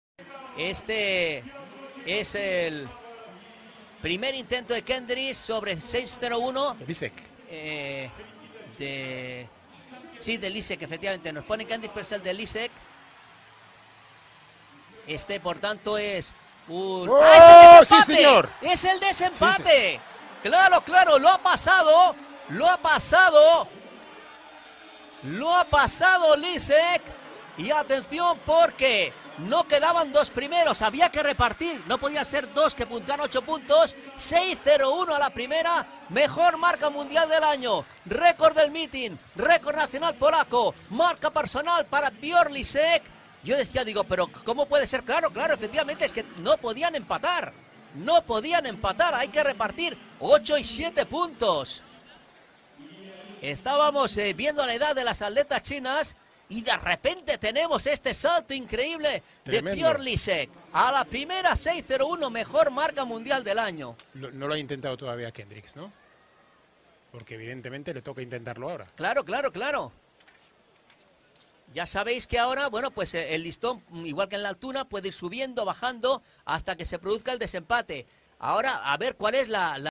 Rekordowy skok Piotra Liska 6.01m
Piotr Lisek, skokiem na  6,01 m na mityngu Diamentowej Ligi w Lozannie pobił rekord Polski w skoku o tyczce